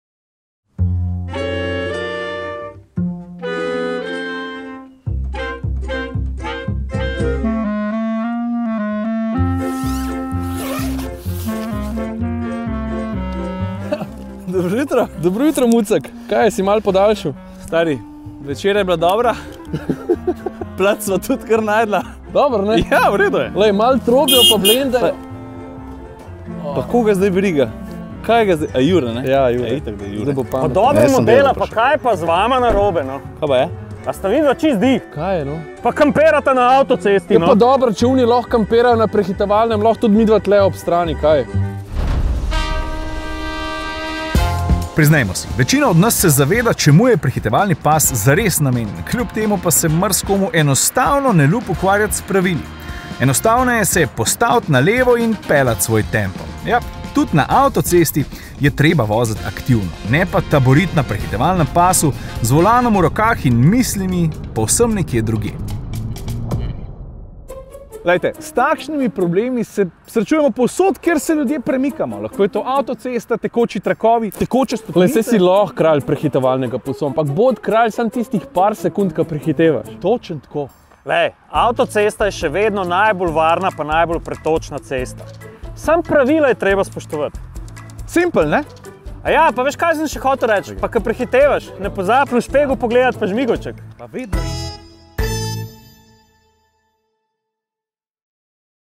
Atmosferci in "kampiranje" na prehitevalnem pasu
Kje pa je vaš najljubši kotiček za kampiranje? Upamo, da ne na prehitevalnem pasu. Kaj si o tem fenomenu mislijo @Atmosferci ? Več